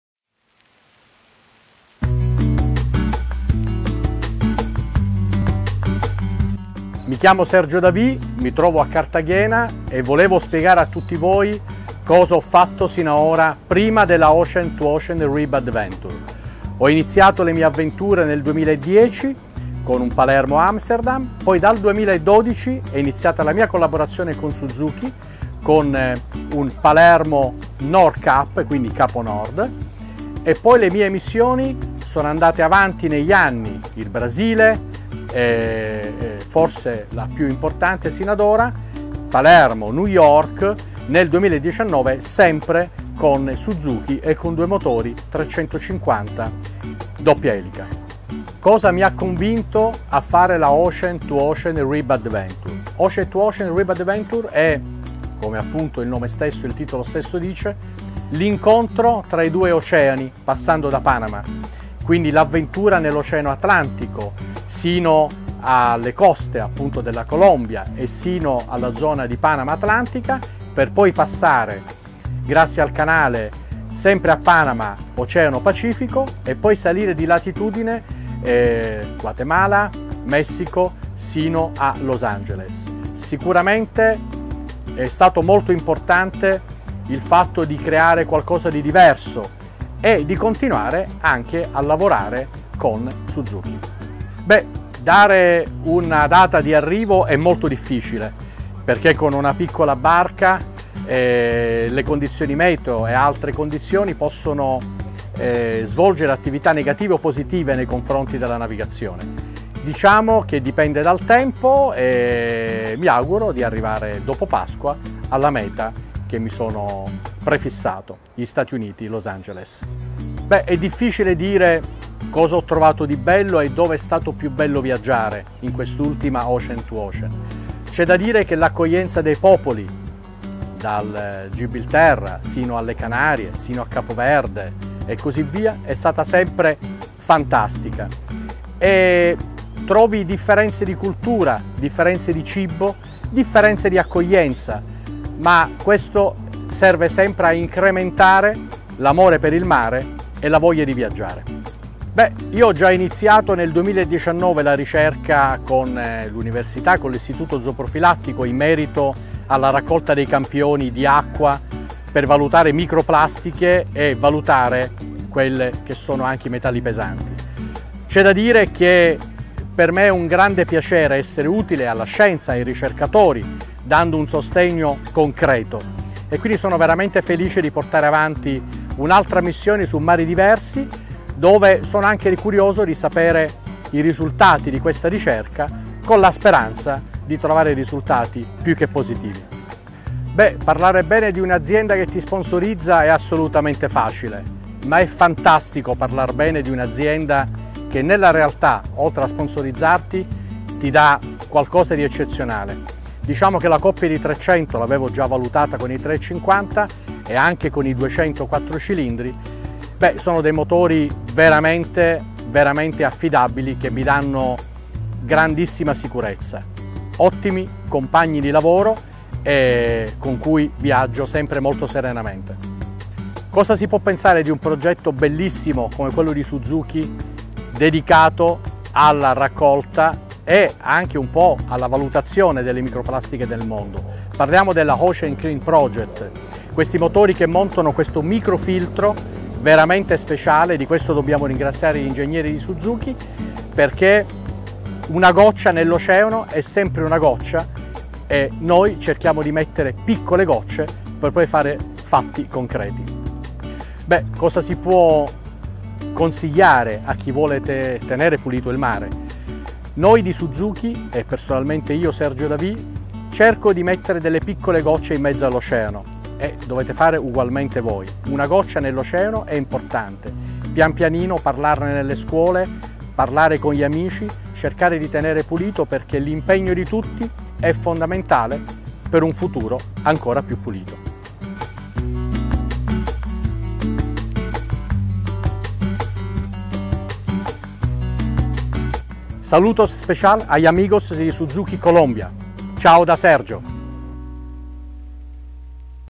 Entrevista Capitán Sergio Davi - Proyecto Suzuki Océano Limpio
Un Capitán que ha navegado los Océanos Atlántico y Pacífico con Dos Motores Fuera de Borda Suzuki y una pasión enorme por los mares y la protección del medio ambiente como nosotros.